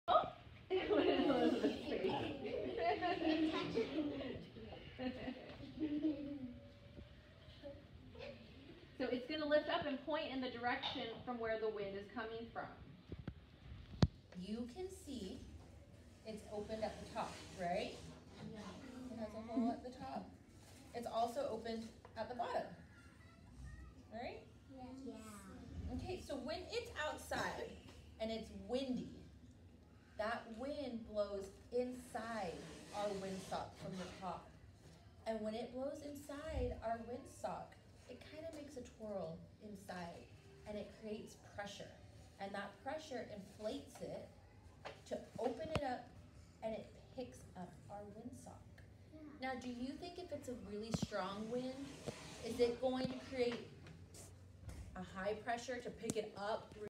Windsock for preschoolers STEM
🎈 What's a Windsock? Our little scientists explored wind speed and direction! By making a class windsock, our kids were able to see how a windsock can help them determine not only the direction the wind is coming from but also its speed!

A windsock is a tool that shows the direction and strength of the wind. Pilots and meteorologists use them, but they can also be beautiful and fun decorations that help our little ones learn about the weather and air around us!
🌟 Why a Windsock for STEM?
Building a windsock combines art, science, and a bit of engineering. It's a fantastic way to engage fine motor skills, understand weather patterns, and spark creativity. Plus, it's just plain fun!
🎨 Let's Get Crafting!
Materials:
A cylindrical object (like an oatmeal container or a long paper roll)
Colorful ribbons, streamers, or fabric
String
Paint, stickers, markers (for decoration)
Glue or tape
Steps:
Decorate the Cylinder: Let your child's imagination run wild! Paint, color, or stick stickers around the cylindrical object. This will be the top of the windsock.
Attach the Tails: Cut the ribbons or streamers and glue or tape them to one end of the cylinder. They should hang down to catch the wind.
Hang It Up: Punch two holes at the top of the windsock (opposite the tails) and attach a string for hanging.
Observe: Hang your windsock outside and watch it flow with the wind! Talk with your preschoolers about how the windsock shows wind direction and strength.